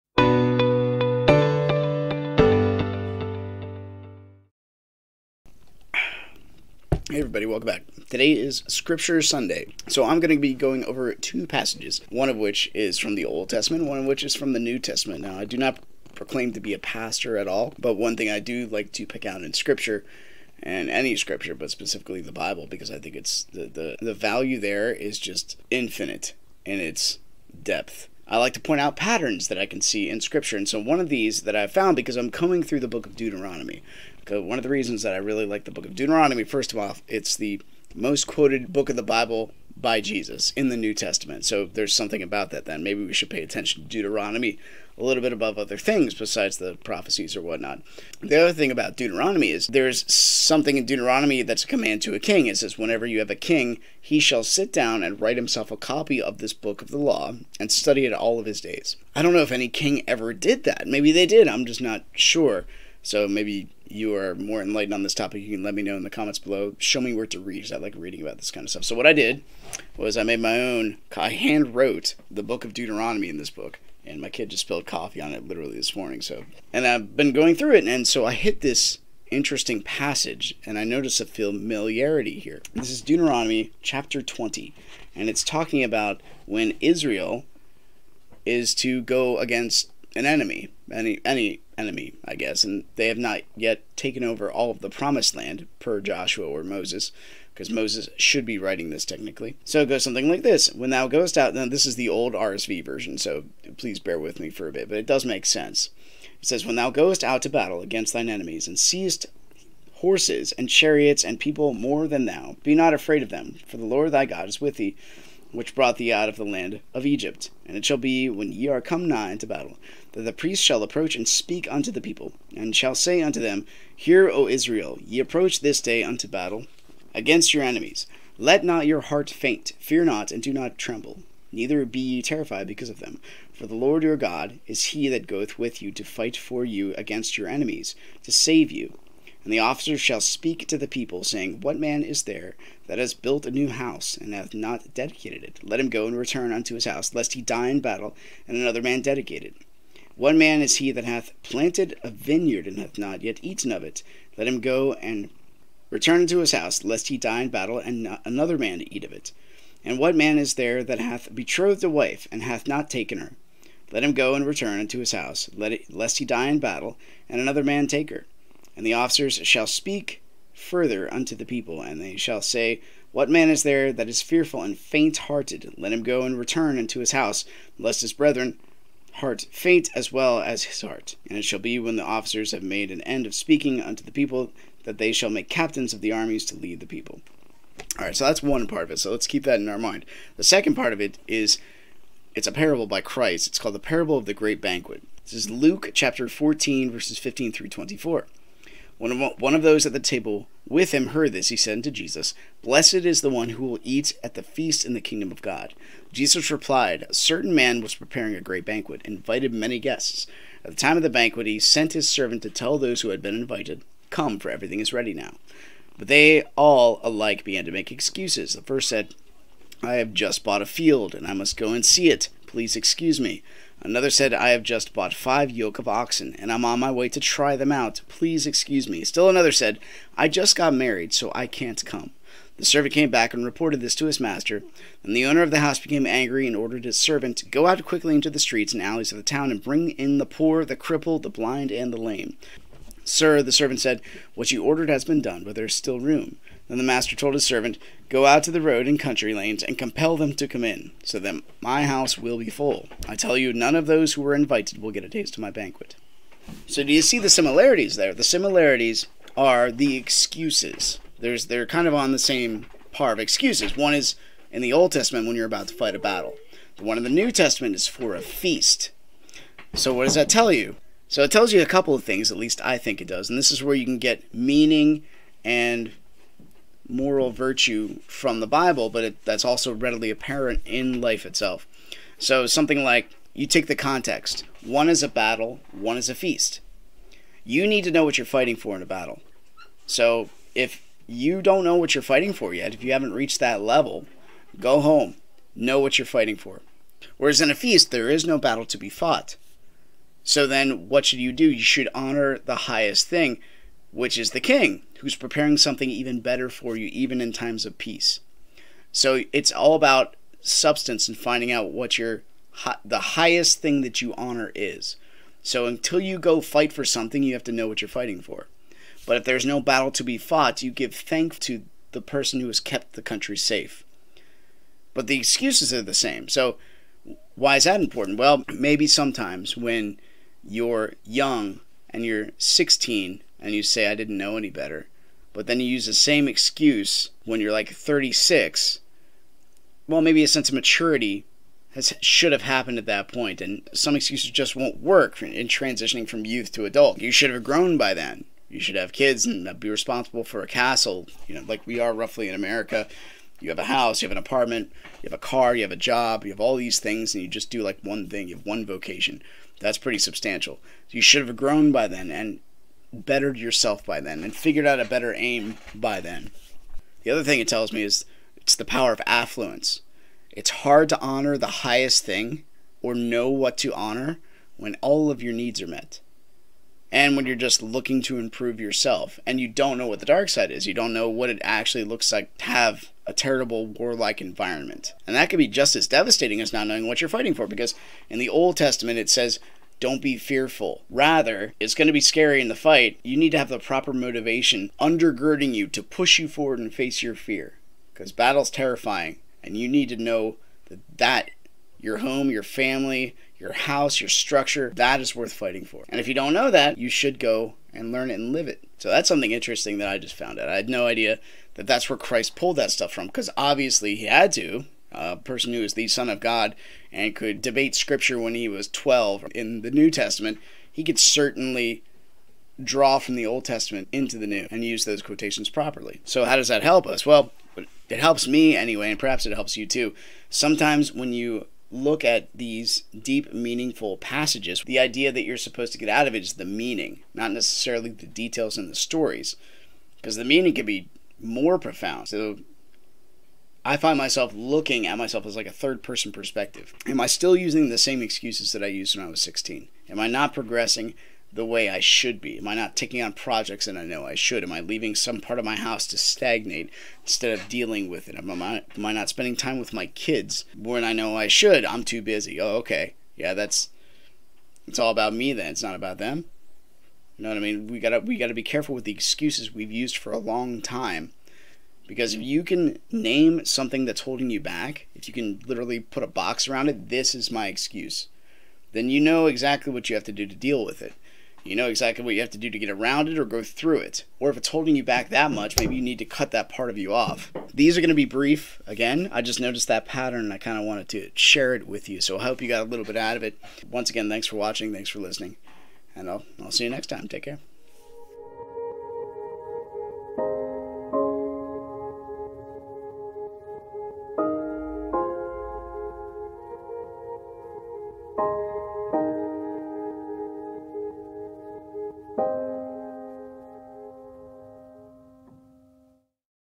The Surprising Relevance of Deuteronomy in the Modern World
A pattern I found in Deuteronomy and Luke which was quite interesting

Luke 14:15-24
Deuteronomy 20:1-9